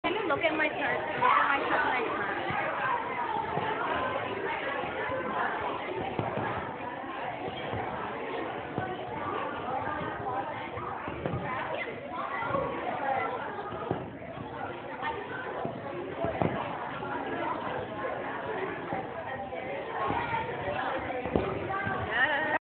My Italian fouettes😊
I was practicing Italian fouettes at my summer intensive and this was the result😁  (sorry for bad quality)